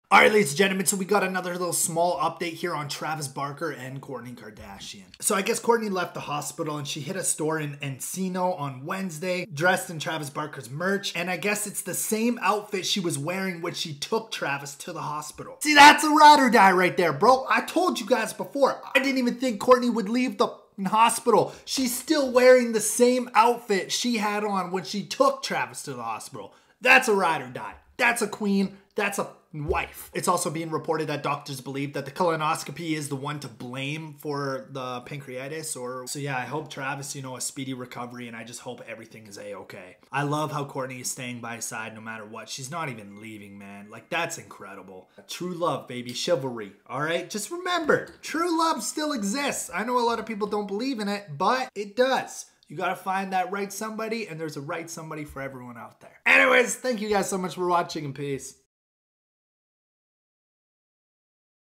Kourtney Kardashian gets CAUGHT!! (aww)
KUWTK star Kourtney Kardashian is staying by her Husband Travis Barker side the WHOLE way while he's in the hospital.. | she was allegedly even caught on camera wearing the same outfit as she had on a couple days ago